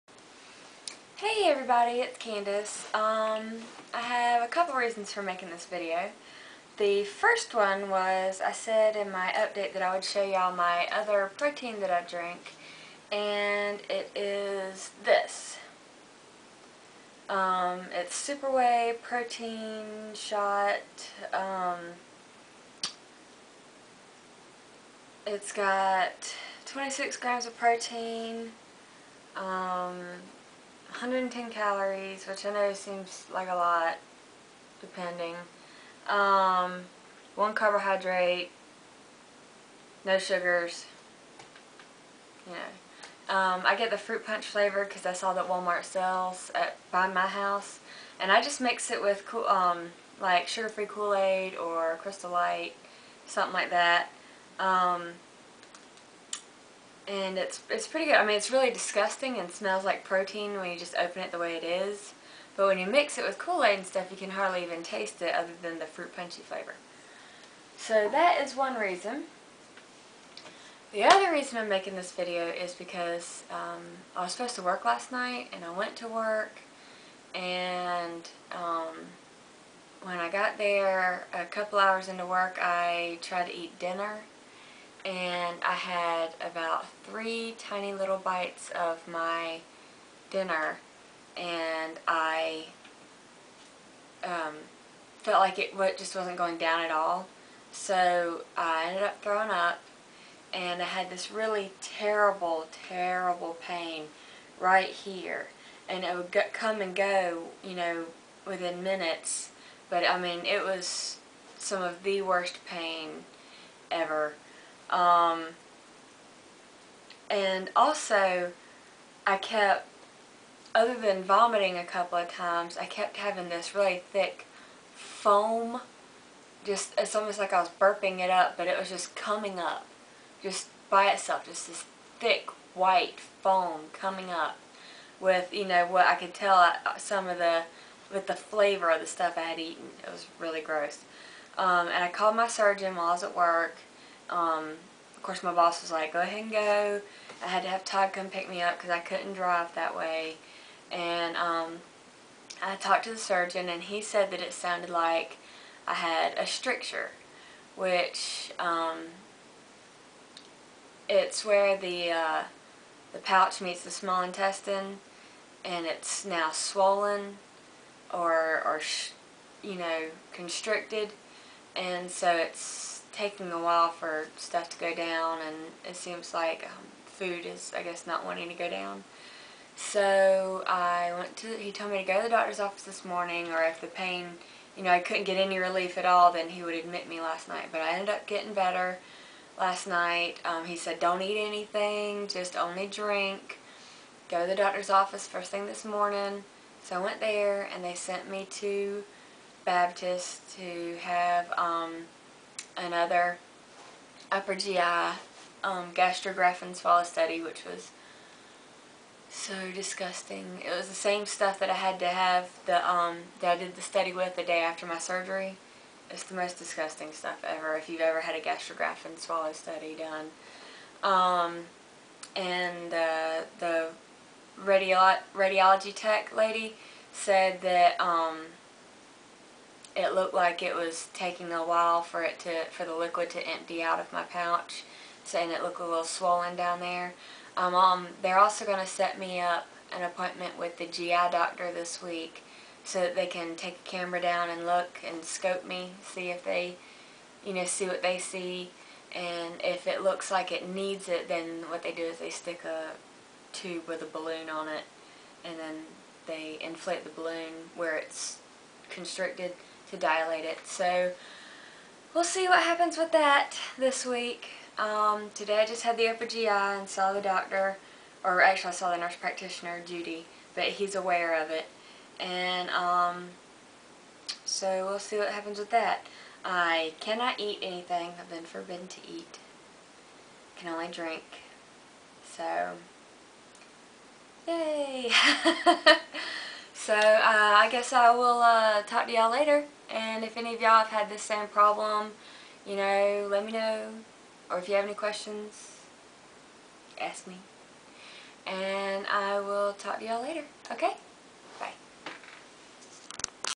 Anastamotic Stricture
I had a few problems last night.  I had to have and UGI sone today and I am not allowed to eat until they figure it all out.